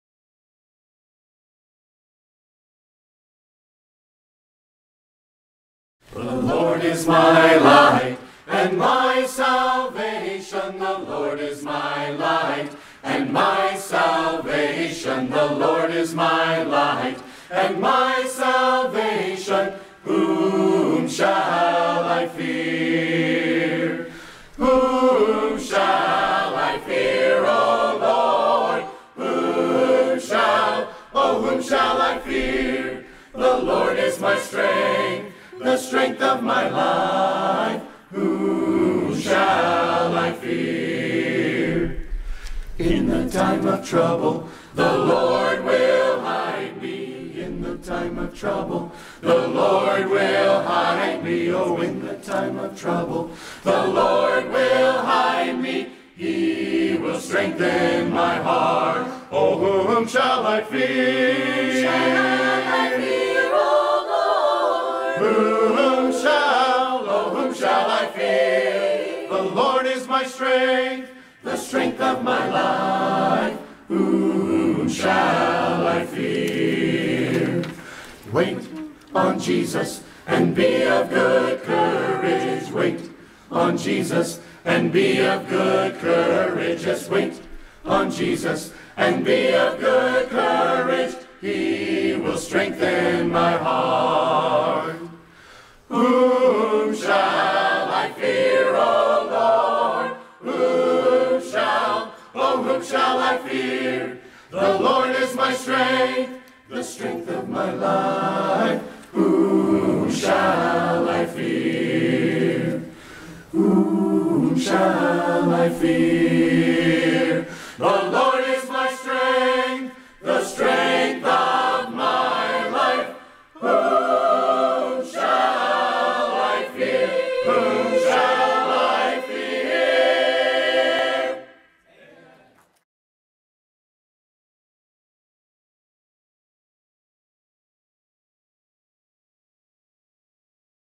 Choir Sunday Jan 26 2020
Special Music Jan 26, 2020 at Cornerstone Baptist Church in Spring Hill Florida.